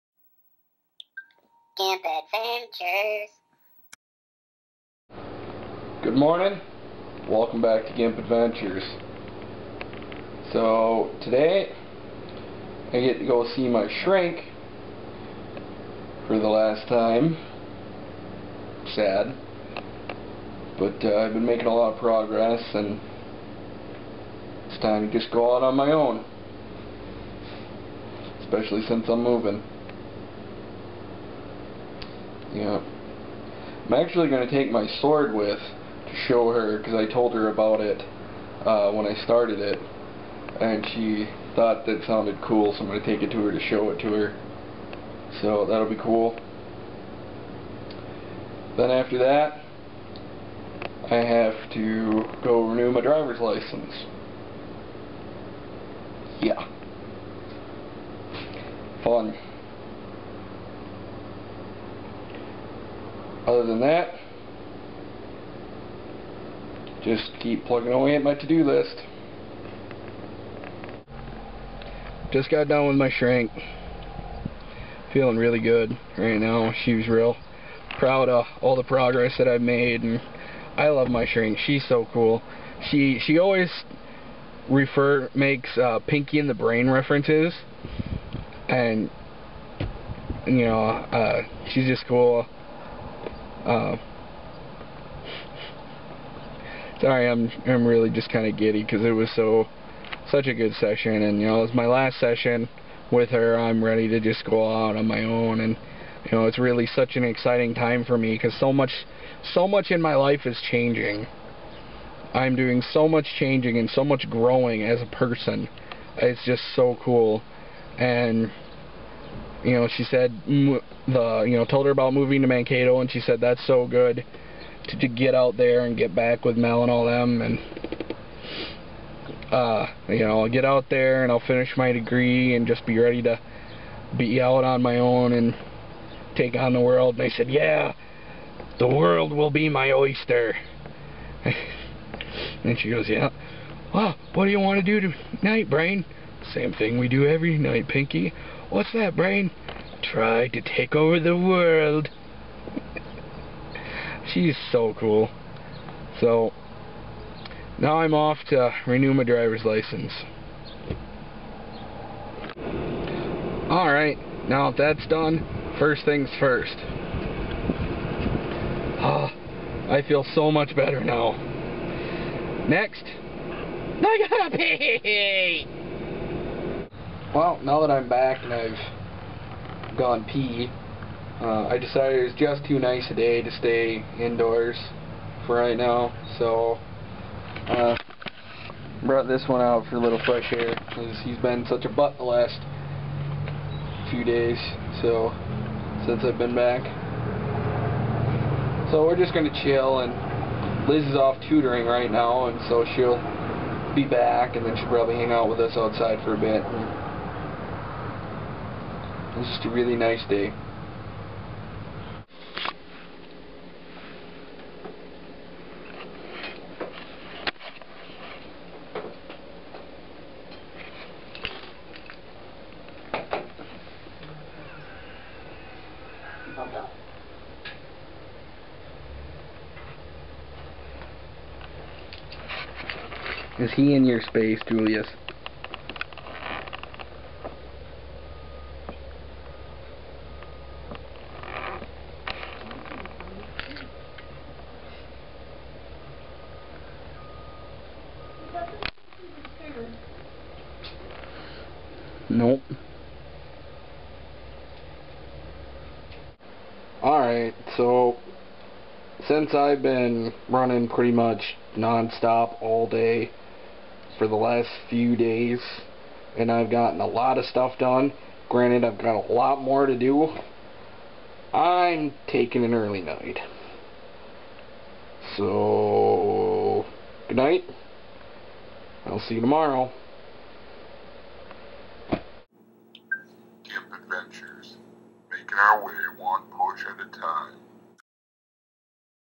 Gee Brain, What Do You Want To Do Tonight? (GA EP113 2:24:2012)
I saw my shrink for the last time. That and checked off some more things on my to do list.